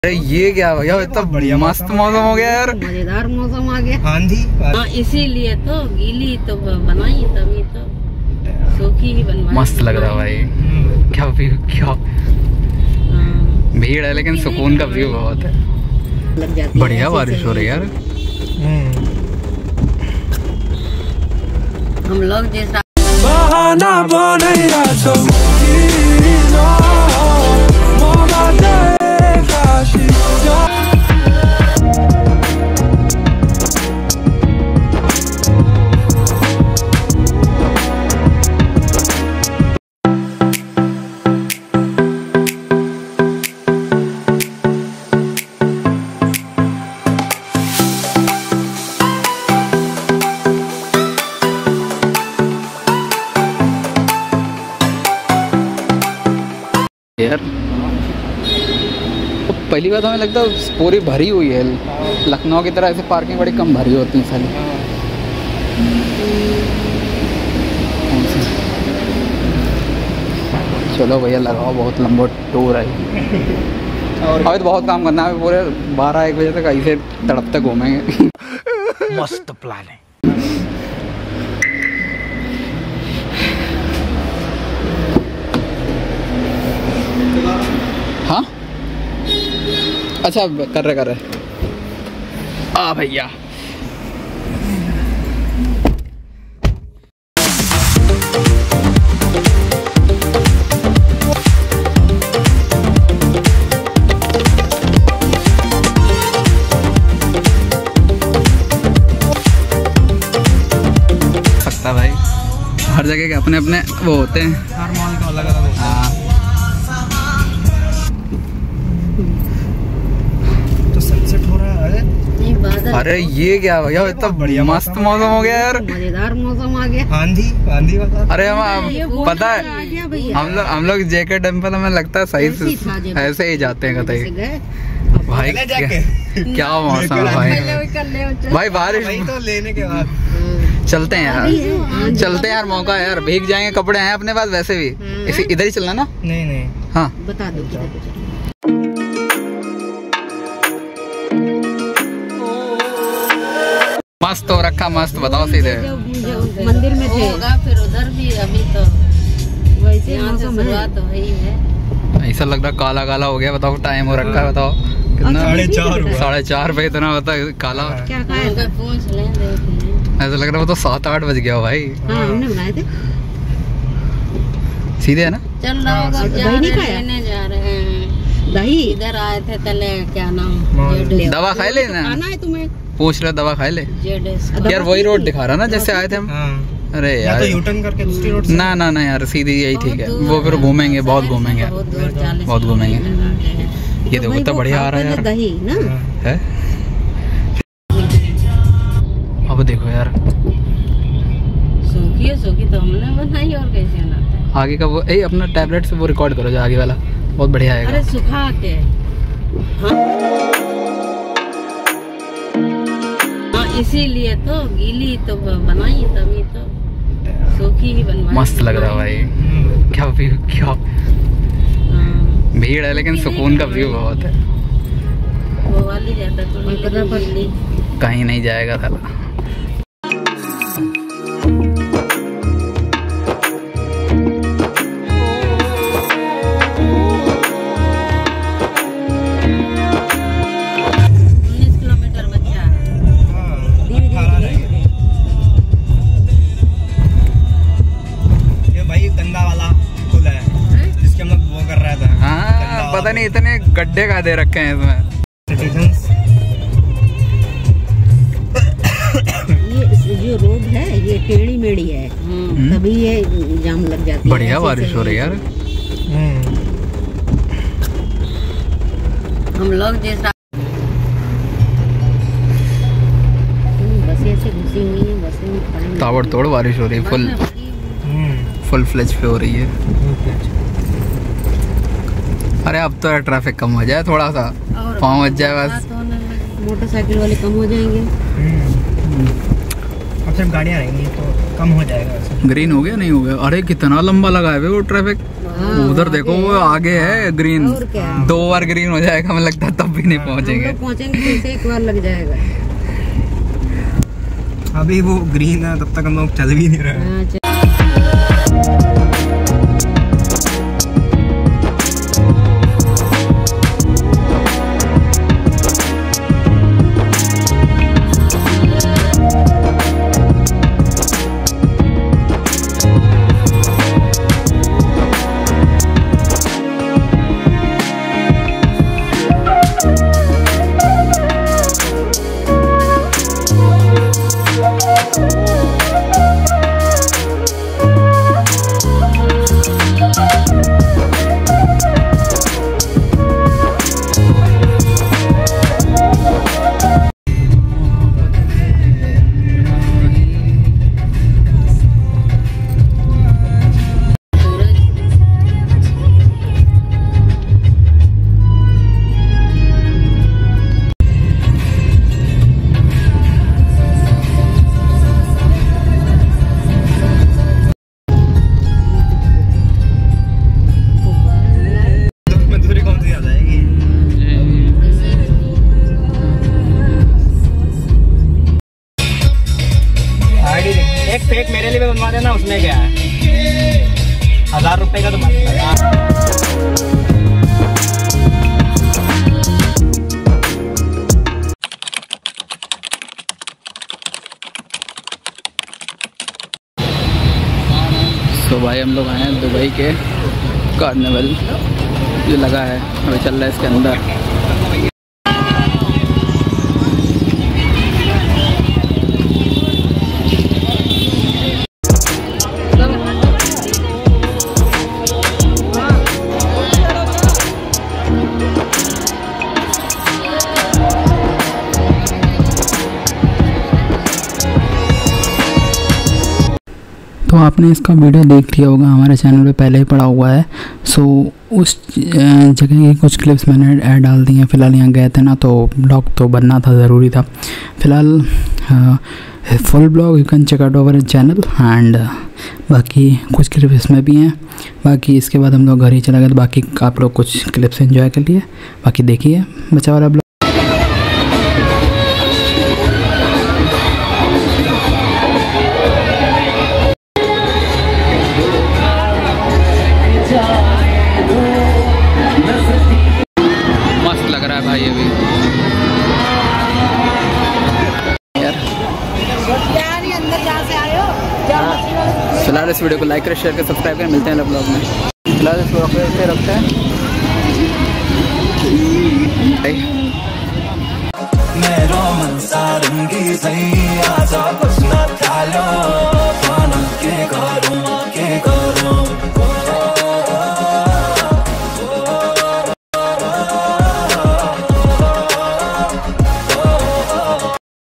Kanpur Se Lucknow NiKal Ayye | Lucknow Me Atte Hi Dekha Dubai Theme Carnival Mela Mohotsav
Hope u enjoyed the vlogs